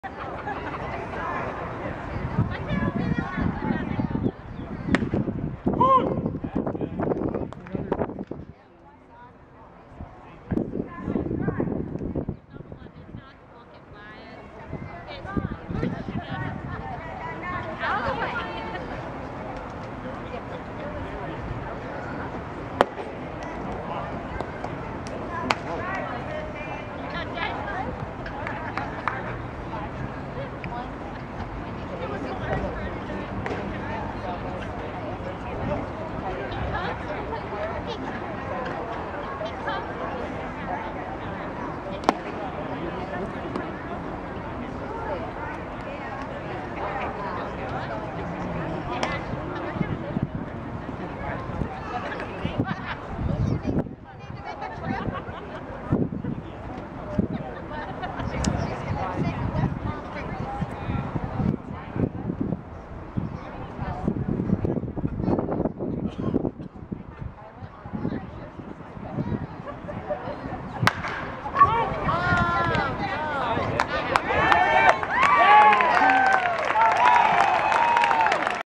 Michael Crouse | AB Batting 2012 Blue Jays
Michael Crouse AB, Dunedin vs Tampa Yankees, April 23, 2012